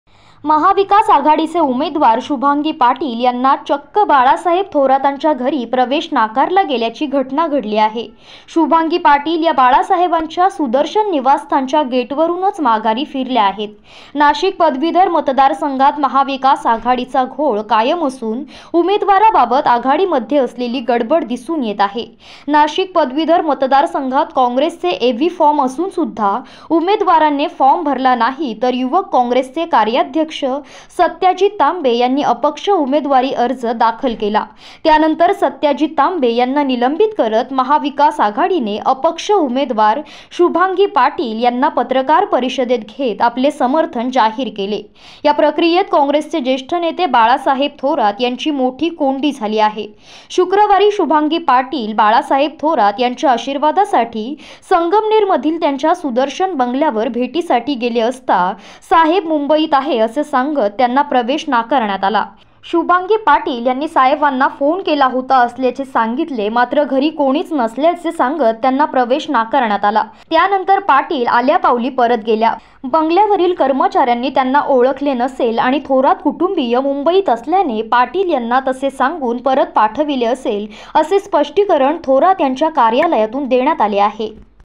शुभांगी पाटील निवडणुकीआधी बाळासाहेब थोरातांच्या घरी, पण फोन करूनही प्रवेश नाकारला !
महाविकास आघाडीचे उमेदवार शुभांगी पाटील निवडणुकीआधी बाळासाहेब थोरातांच्या घरी, पण फोन करूनही प्रवेश नाकारला !